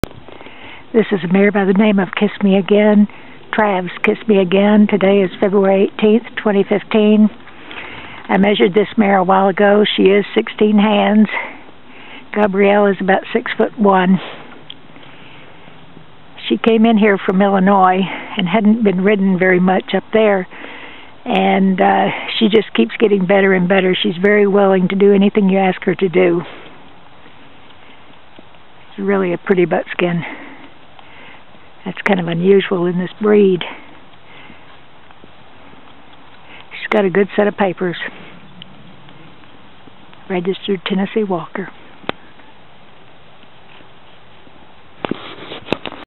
FOR SALE - TRAV'S KISS ME QUICK RSW #20702872
FOR SALE - TRAV'S KISS ME QUICK RSW #20702872 - Beautiful buckskin Tennessee Walking Horse mare - 7 years old - is very gentle and has a great mind - 16 hands - with Pusher and Ebony Masterpiece breeding.  Sweet natured and easy to work with.  Needs some regular riding and will make someone a great trail horse. Foaled 05/14/2007. Horse is located in Texas. Priced at $3,800.